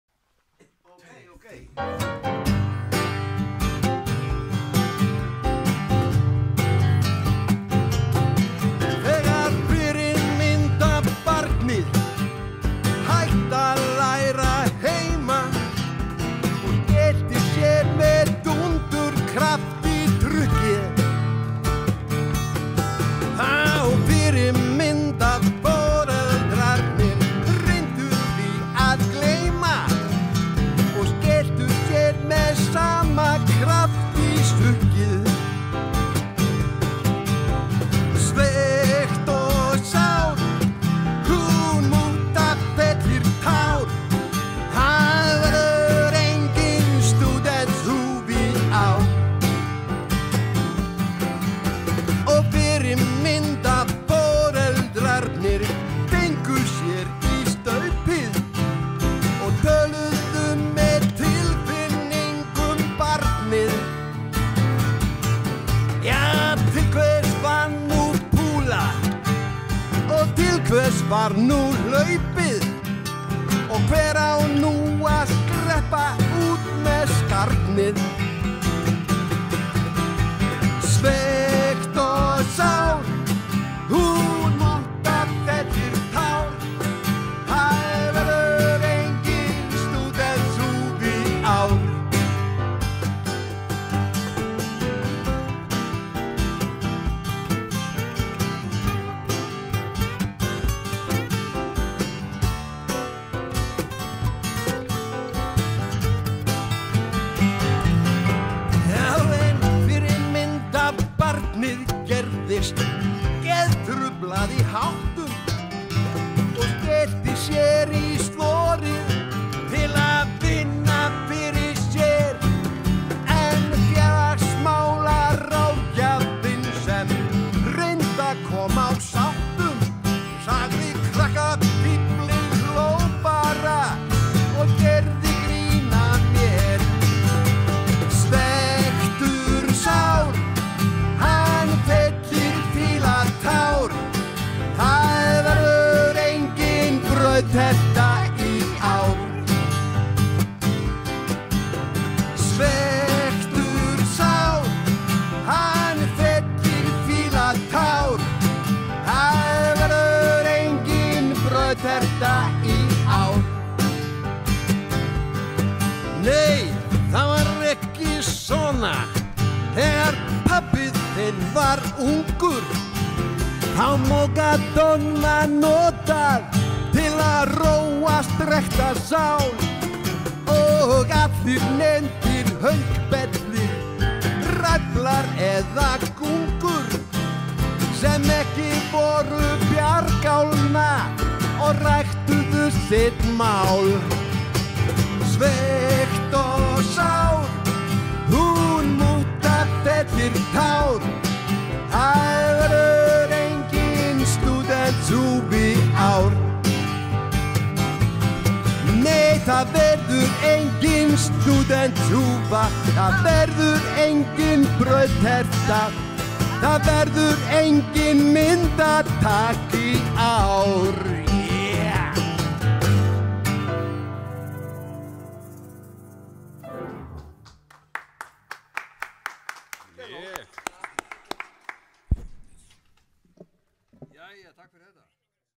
Í tilefni af minningar og afmælistónleikum Péturs Kristjánssonar - Seinni hluti
Í tilefni af minningar og afmælistónleikum Péturs Kristjánssonar komu Bjartmar Guðlaugsson og félagar í Stúdíó 12 og tóku nokkur vel valin lög.